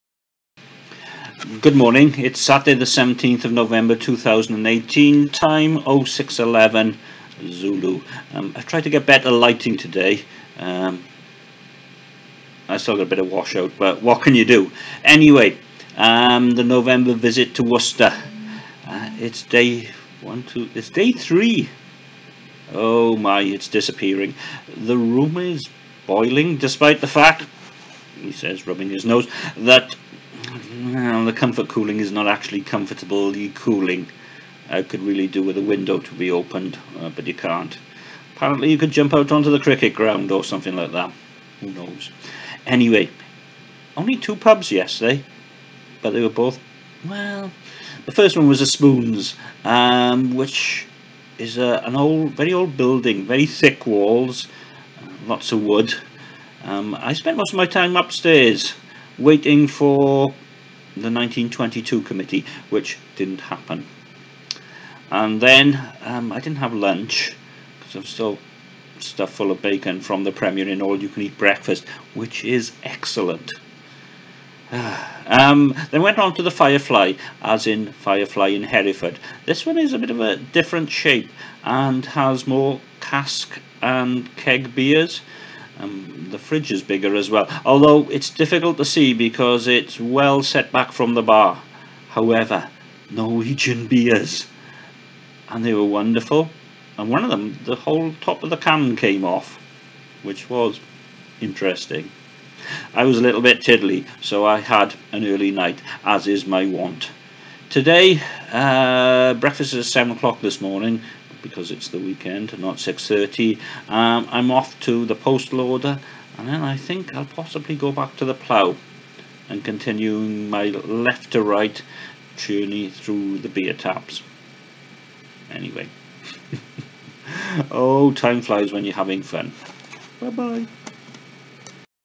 The Hot Room Saturday 20181117
Hoping for slightly better lighting and a cooler room. I think I've succeeded on the first but not on the latter. A look back on my visits to the Crown and Firefly Worcester yesterday and my plans for Saturday.